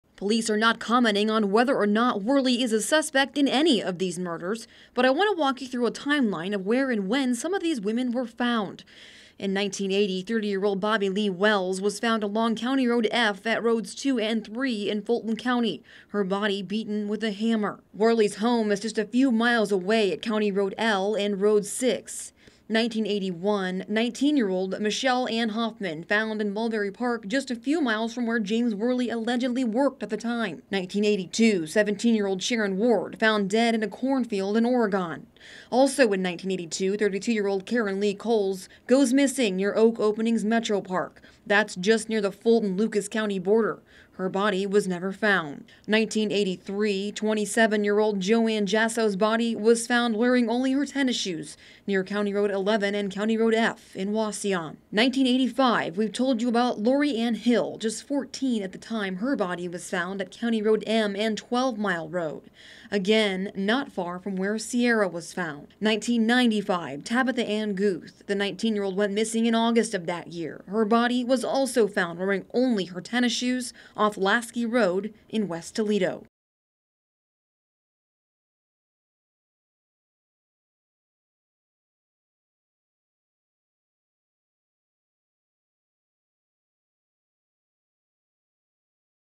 Timeline: Fulton Co. women who have gone missing; many hold hope ties can be found in Worley case
This story aired on Aug. 23, 2016: Now, police are not commenting on whether or not Worley is a suspect in ANY of these murders, but I want to walk you through a timeline of where and when some of these women were found.
In 1980, 30-year-old Bobbie Lee Wells was found on County Road F at Roads 2 and 3 in Fulton County, her body beaten with a hammer.
Worley's home is just a few miles away at County Road L and Road 6.
1981: 19-year-old Michelle Ann Hoffman found in Mulberry Park just a few miles from where James Worley allegedly worked at the time.
1982: 17-year-old Sharon Ward found dead in a cornfield in Oregon.
Also in 1982, 32-year-old Karen Lee Kohls goes missing near Oak Openings Metropark; that's just near the Fulton-Lucas County border. Her body was never found.
1983: 27-year-old Joann Jaso's body was found wearing only her tennis shoes near County Road 11 and County Road F in Wauseon.
1985: We've told you before about Lori Ann Hill just 14 at the time her body was found at County Road M and 12-mile road. Again, not far from where Sierah was found.
1995: Tabitha Ann Guth. The 19-year-old went missing in August of that year; her body was also found wearing only tennis shoes off Laskey Road in West Toledo.